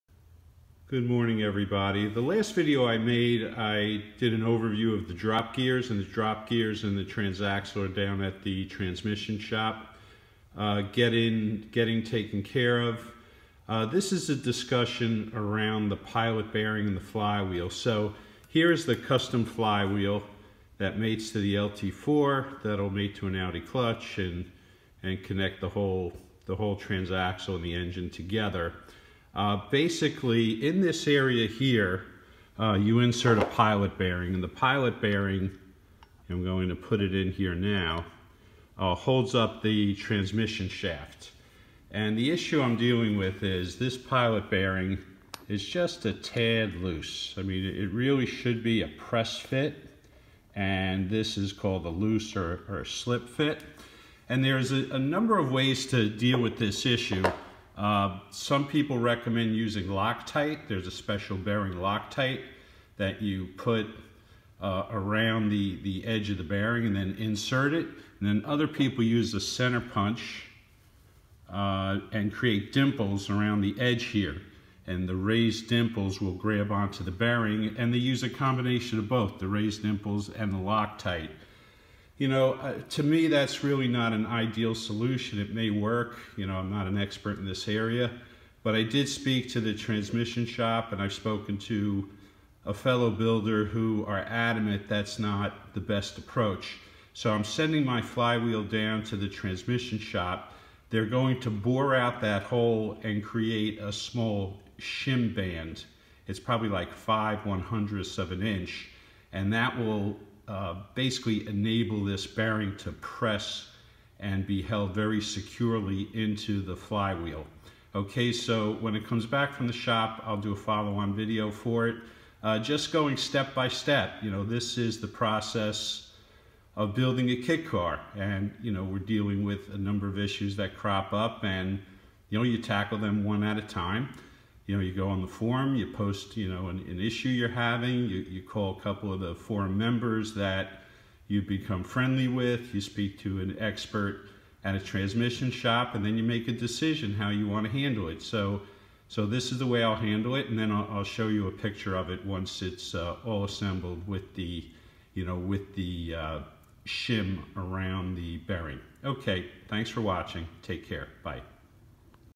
Superlite SLC Build 010 - Pilot Bearing Fitment Part 1
Discussion on different approaches to firming up a slip fit pilot bearing.